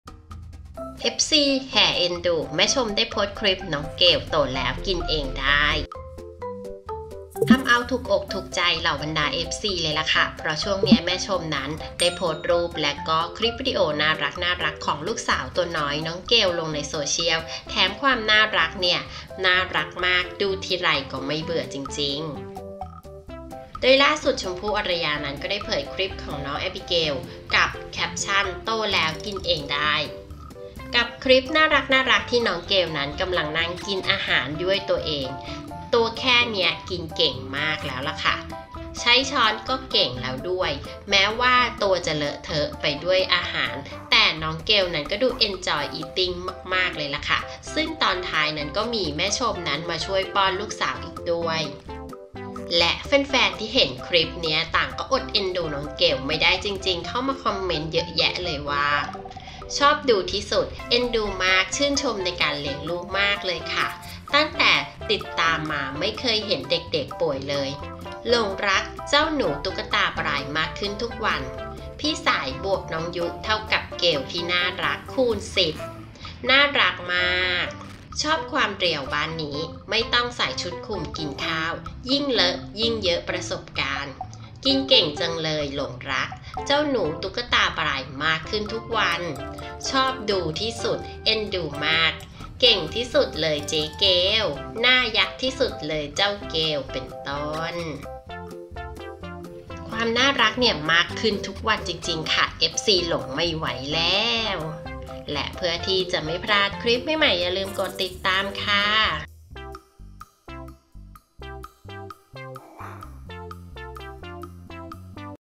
Fc แห่เอ็นดูมาก "แม่ชม" โพสต์คลิป "น้องเกล" โตแล้ว กินเองได้
เอ็ฟซีแห่เอ็นดูมาก "แม่ชม" โพสต์คลิป "น้องเกล" โตแล้ว กินเองได้

ทำเอาถูกใจเอฟซีจริงๆ เพราะช่วงนี้ แม่ชม โพสต์รูปโพสต์รูปและคลิปน่ารักๆ ของ น้องเกล ลูกสาวตัวน้อยลงโซเชียลเยอะมาก แถมน่ารักมาก ดูทีไรก็ไม่มีเบื่อจริงๆ

โดยล่าสุด ชมพู่ อารยา ได้โพสต์คลิป น้องเกล กับแคปชั่นว่า "โตแล้ว กินเองได้" กับคลิปน่ารักๆ ที่ น้องเกล กำลังนั่งกินอาหารด้วยตัวเอง ตัวแค่นี้กินเก่งมได้แล้ว ใช้ช้อนเก่งแล้วด้วย แม้ตัวจะเลอะเทอะไปด้วยอาหาร แต่ดู น้องเกล จะเอ็นจอยอีทติ้งมาก ซึ่งตอนท้ายก็มี แม่ชม ที่มาช่วยป้อนลูกสาวด้วย

และแฟนๆ ที่เห็นคลิปก็อดเอ็นดู น้องเกล ไม่ได้จริงๆ คอมเมนต์กันเยอะเลย เช่น ชอบดูที่สุด เอ็นดูมากกก,ชื่นชมในการเลี้ยงลูกมากเลยค่ะ ตั้งแต่ติดตามมาไม่เคยเห็นเด็กๆป่วยเลย,หลงรัก เจ้าหนูตุ๊กตาบลายธ์มากขึ้นทุกวัน,พี่สาย+น้องยุ = เกลที่น่ารักคูณ10,น่ารักมากกกกกกกก